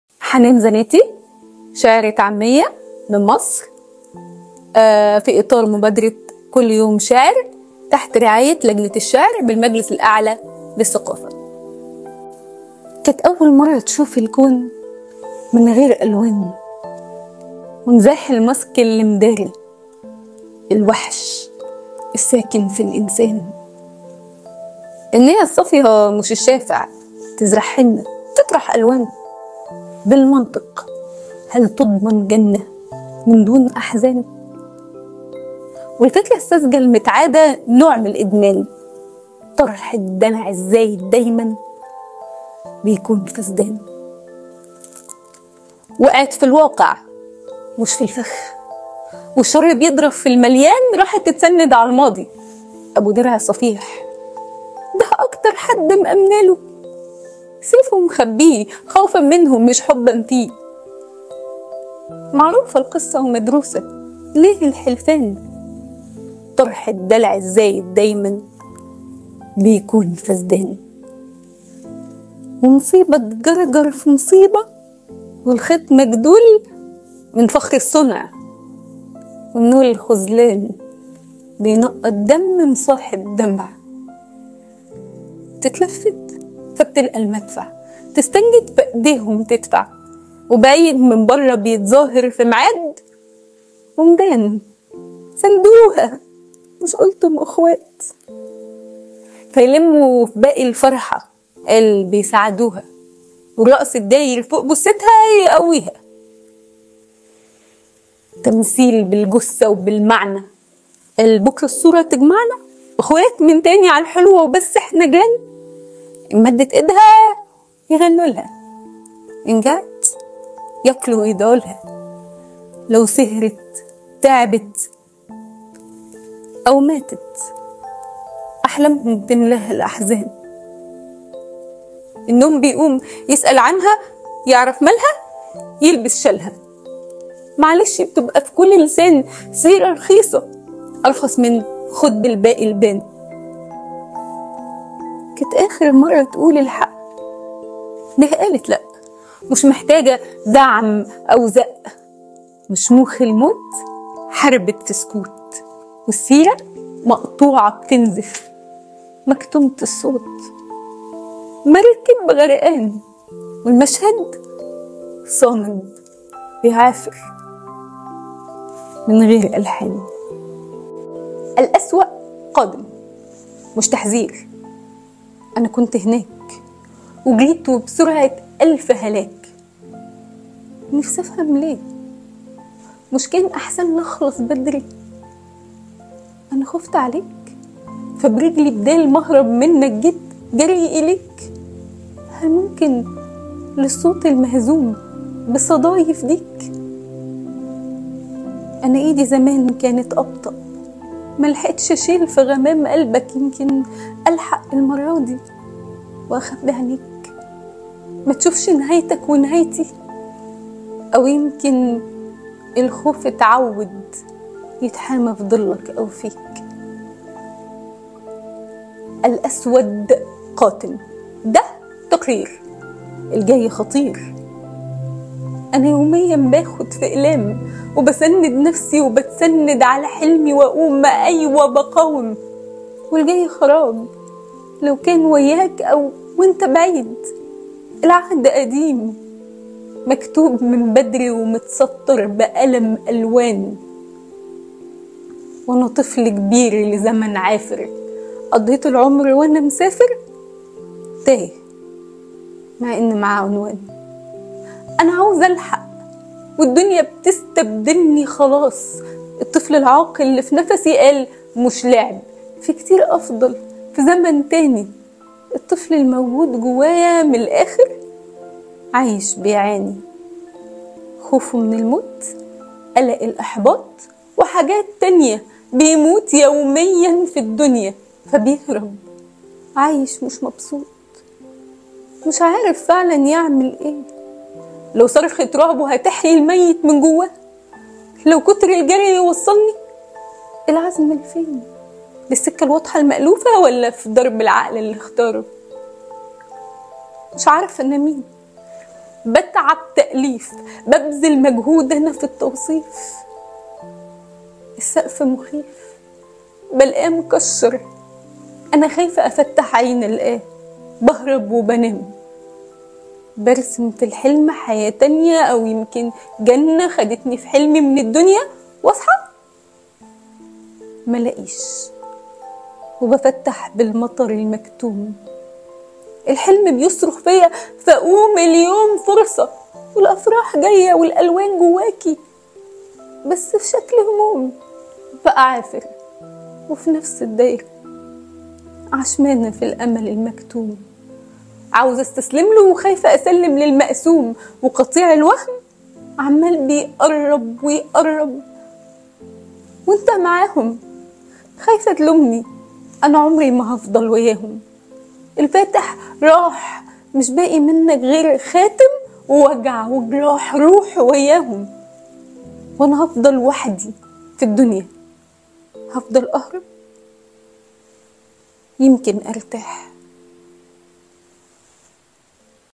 الشاعرة/ حنان زناتي - مصر- قصيدتين "الفيلم العربي-المسيخ الدجال"
وزارة الثقافة
المجلس الأعلى للثقافة - لجنة الشعر
الشاعرة/ حنان زناتي - مصر
تقدم قصيدتين بعنوان"الفيلم العربي-المسيخ الدجال"
الأربعاء 6 نوفمبر2024 - الساعة السابعة مساء
تحت رعاية الأستاذ الدكتور/ أحمد فؤاد هنو - وزير الثقافة المصرية
يتشرف الأستاذ الدكتور/ أسامة طلعت - الأمين العام للمجلس الأعلى للثقافة و الدكتور/ يوسف نوفل - مقرر لجنة الشعر بالمجلس بدعوة سيادتكم لمتابعة فعاليات مبادرة كل_يوم_شاعر الذي تنظمه لجنة الشعر بالمجلس
ونلتقى خلالها مع الشاعرة/ حنان زناتي - مصر
** الأفكار التي تتضمنها كافة قراءات المشاركين بالمبادرة لا تعبر بالضرورة عن رأي المجلس الأعلى للثقافة.**
تقبلوا تحيات فريق عمل أمانة المؤتمرات